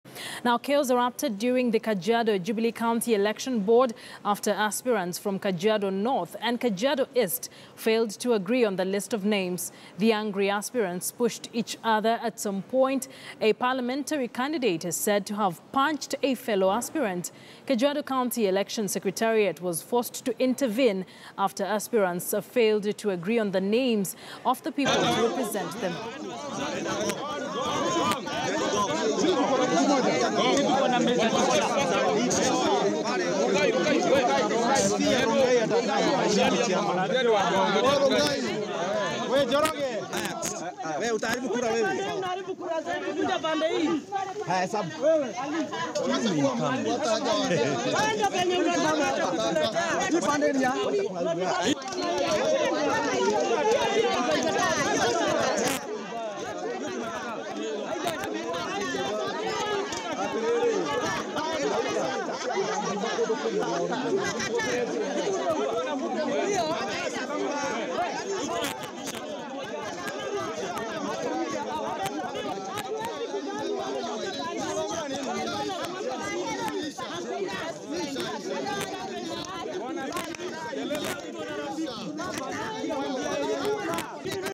Chaos marrs Kajiado Jubilee county elections board after aspirants fail to reach concensus
Chaos erupted during the Kajiado Jubilee county election board after aspirants from Kajiado north and Kajiado east failed to agree on the list of names. The angry aspirants pushed each other and at some point a parliamentary candidate is said to have punched  a fellow aspirant . Kajiado County election secretariat was forced to intervene after aspirants failed to agree on the names of people to represent them. This is happening despite Jubilee party setting up   a disciplinary committee to deal with its chaotic nomination process saying the committee will deal firmly with those seeking to disrupt the party’s systems.